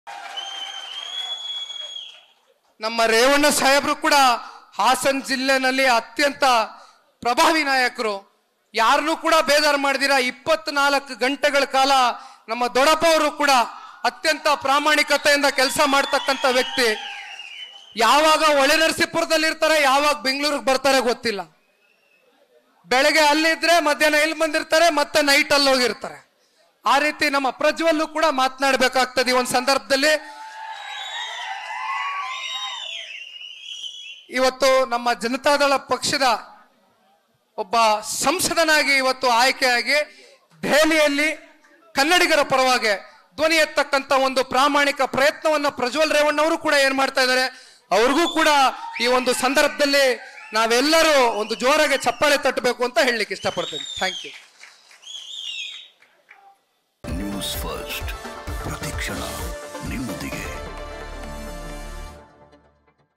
HD Revanna, MP Prajwal Revannaನ ಬಗ್ಗೆ ಹಾಡಿ ಹೊಗಳಿದ Nikhil Kumaraswamy | Jaguar 5th Year Anniversary
HD Revanna, MP Prajwal Revannaನ ಬಗ್ಗೆ ಹಾಡಿ ಹೊಗಳಿದ Nikhil Kumaraswamy | Jaguar 5th Year Anniversary | NewsFirst Kannada